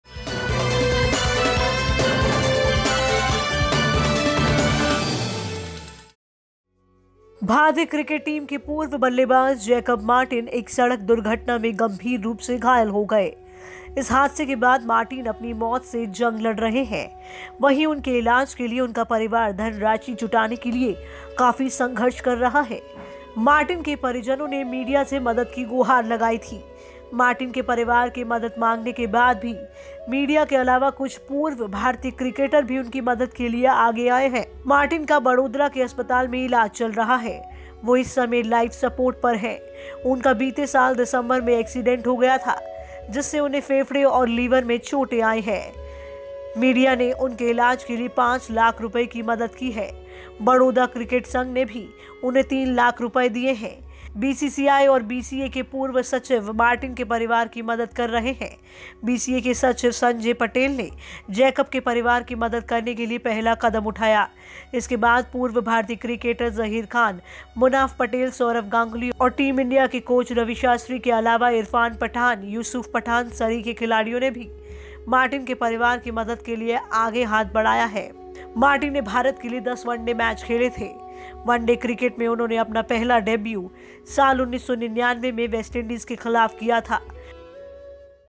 Jacob Martin की मदद के लिए सामने आए Saurav Ganguly समेत ये सभी खिलाड़ी
भारतीय क्रिकेट टीम के पूर्व बल्लेबाज़ जैकब मार्टिन एक सड़क दुर्घटना में गंभीर रुप से घायल हो गए। इस हादसे के बाद मार्टिन अपनी मौत से जंग लड़ रहे हैं वहीं उनके इलाज के लिए उनका परिवार धनराशि जुटाने के लिए संघर्ष कर रहा है। मार्टिन ने परिजनों ने बीसीसीआइ से मदद की गुहार लगाई थी। मार्टिन के परिवार के मदद मांगने के बाद बीसीसीआइ के अलावा कुछ पूर्व भारतीय क्रिकेटर भी उनकी मदद के लिए आगे आए हैं।

VISHESH MEDIA